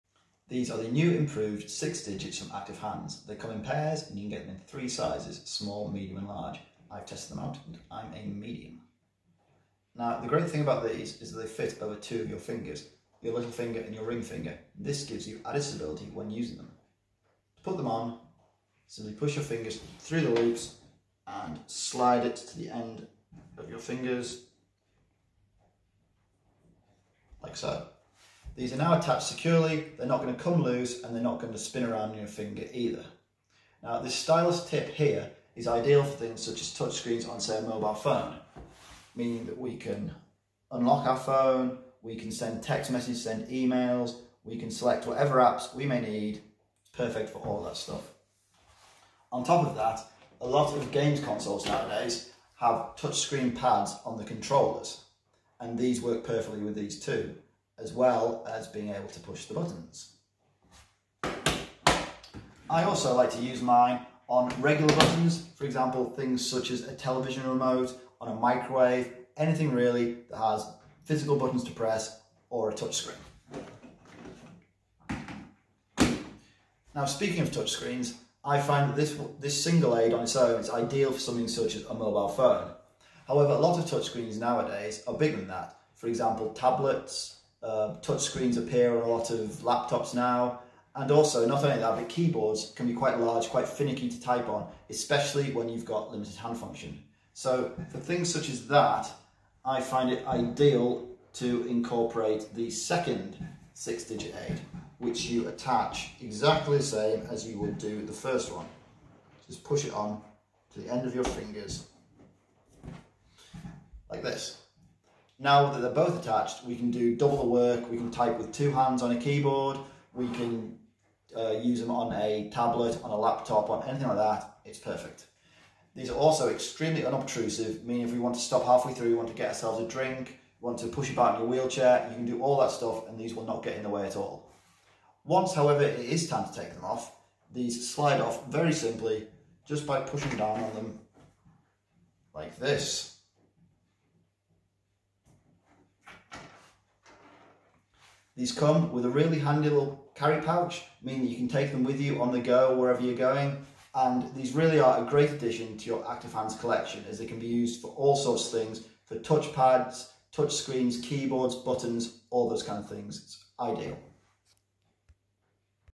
Sixth Digit 2 | The Active Hands Company Ltd
The Sixth Digit 2 is a wearable stylus that fits on your last two fingers allowing you to use touch screens and press buttons. It is can be used on touch screens like phones, tablets and appliance controls as it is made of a connective fibre-filled material.

The wearable stylus has two rings for stability. Put it on by sliding the rings over your ring and smallest finger with the stylus tip pointing away from your hand. There is a step in the design that matches the step in your hand to make it more comfortable to wear.

The Sixth Digit 2 comes with three sizes of device: small, medium and large. And you get all three in a stylish natural cotton bag. It is difficult to measure which size you need so we provide all 3 so you can find the ones that best fit you. You may need a different size for each hand. Once you have found the best size for you, attach the two stylus tips.